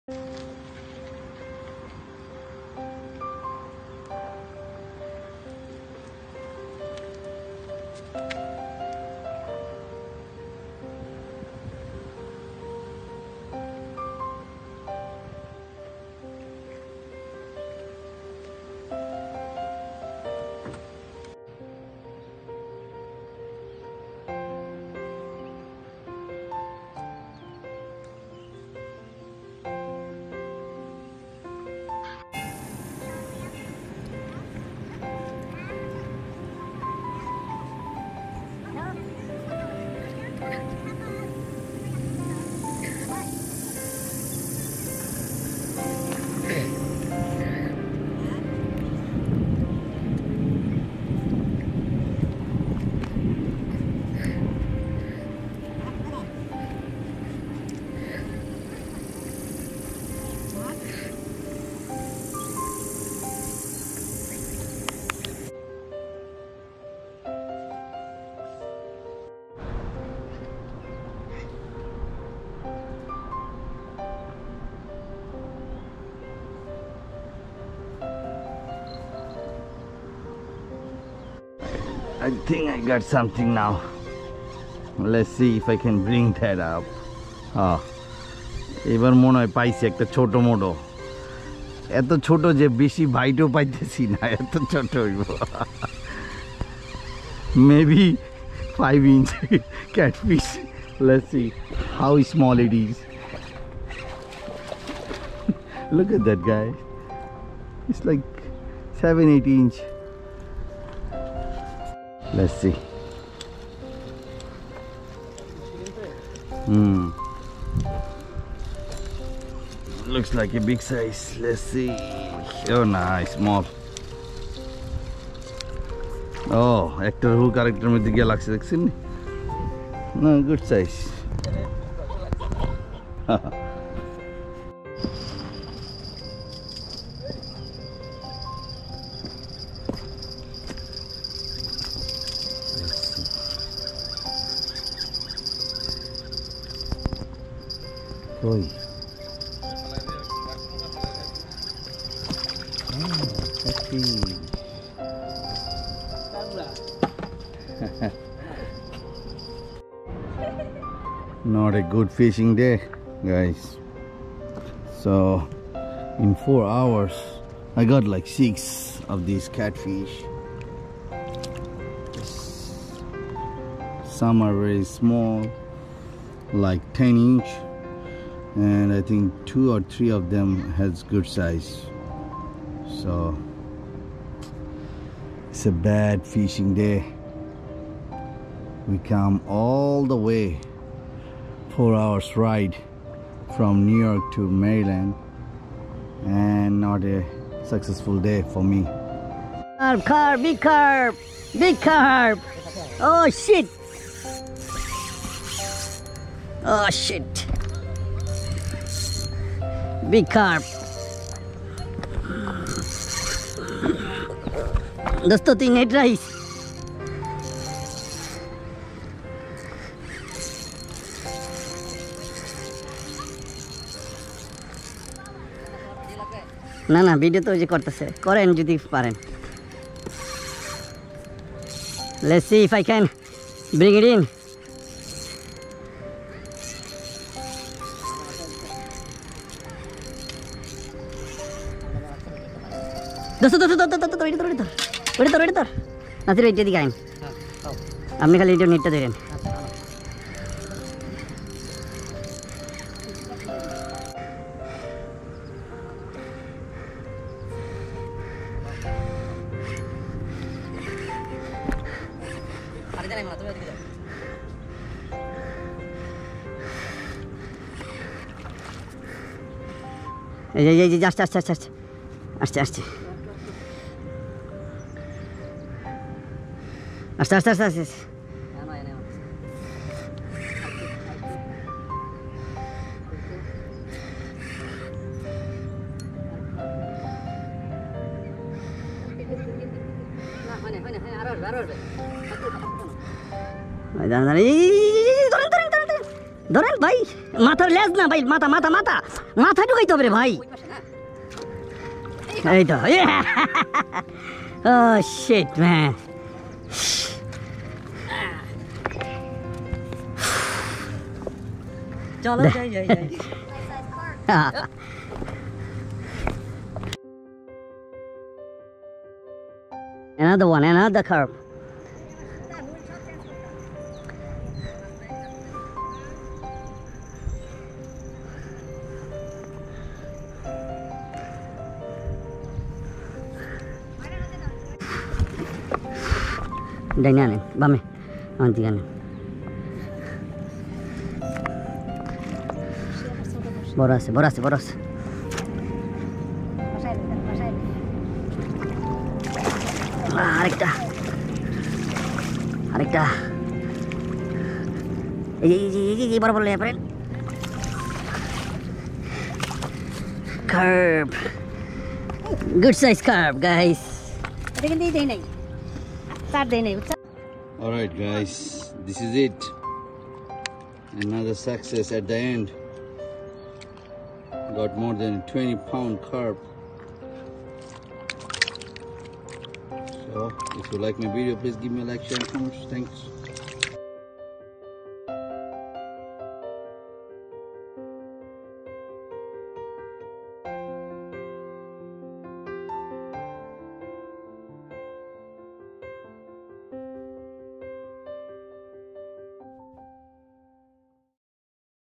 Carp N Cat Fishing in Maryland!!! July 2024 Fishing
Went to Maryland with some of my friends to explore my old fishing spots. In the beginning we thought we will get skunked but after 4 hour of fishing we keep getting bites and landed in but for the extreme heat wave we couldn't continue that day.
It helps my channel grow and allows me to continue making more videos for more refreshment with full power.